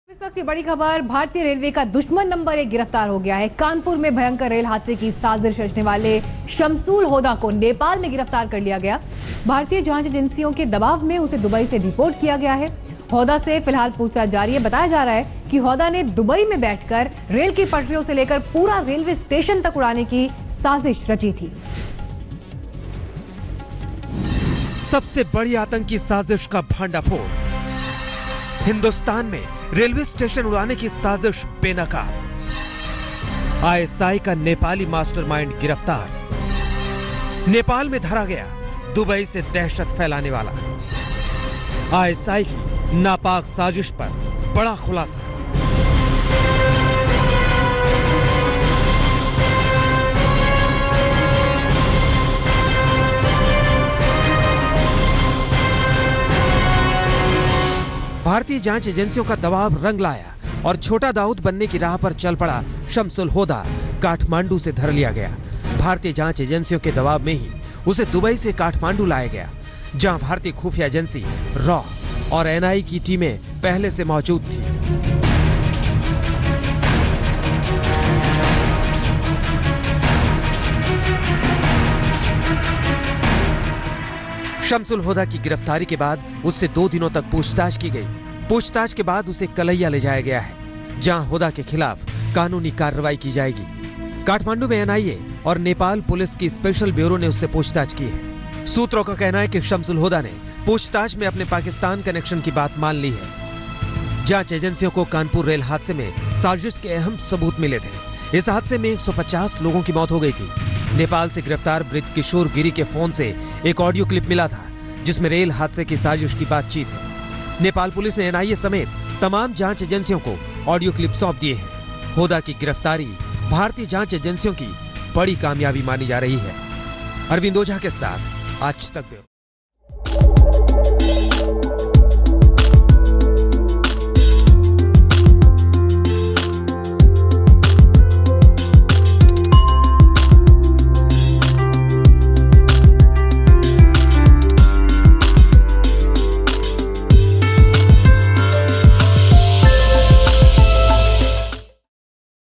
ISI's Agent Shamshul Huda, Mastermind Of Kanpur's Train Tragedy Nabbed From Kathmandu
Shamshul Huda is accused of working for ISI. Huda was involved in the Kanpur train derailment.